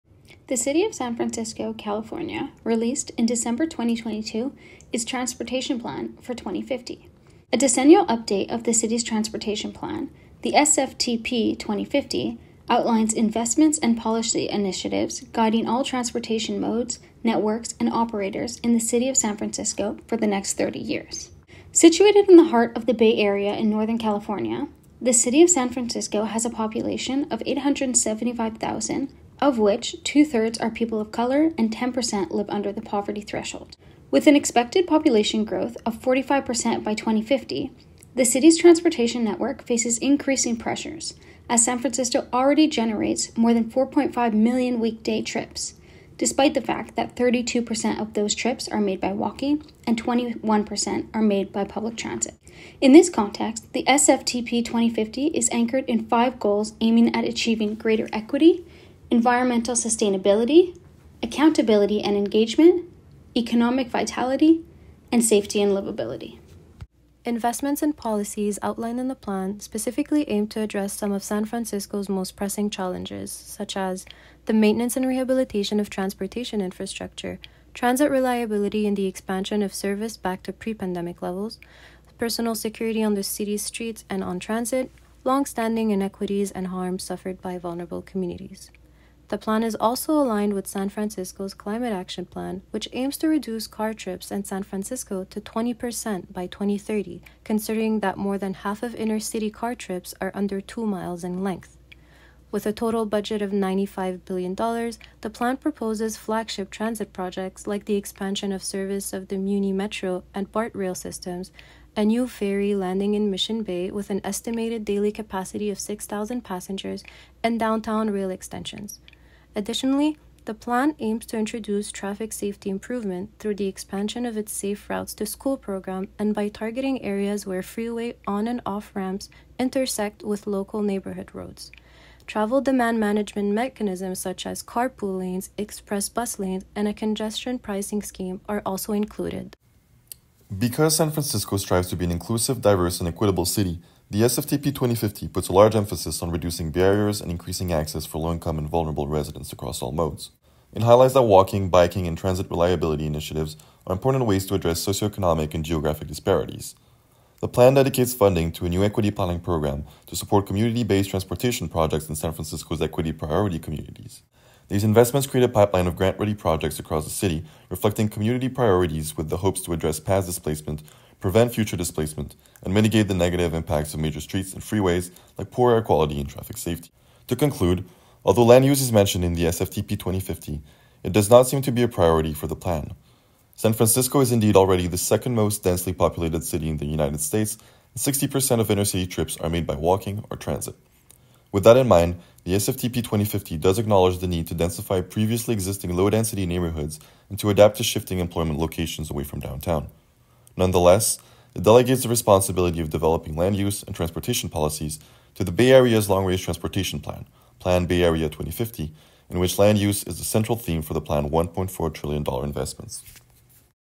San Francisco Transportation Plan 2050
Diving into the future of urban mobility, this video presents an analysis of the San Francisco Transportation Plan 2050. This critical document outlines the investments and policy initiatives set to shape the city's transportation infrastructure over the next three decades. Positioned as the second most densely populated city in the United States, San Francisco anticipates a population increase of 45% by 2050, placing unprecedented demands on its transportation network, which already accommodates over 4.5 million weekday trips. Produced by Cassidy Murray, Sarah Balaghi, and Renaud Delisle for the McGill URBP 619 course on Land Use and Transport, this analysis aims to provide a comprehensive overview of how San Francisco plans to address the evolving challenges and opportunities within its transportation system, ensuring a sustainable and efficient future for all its residents.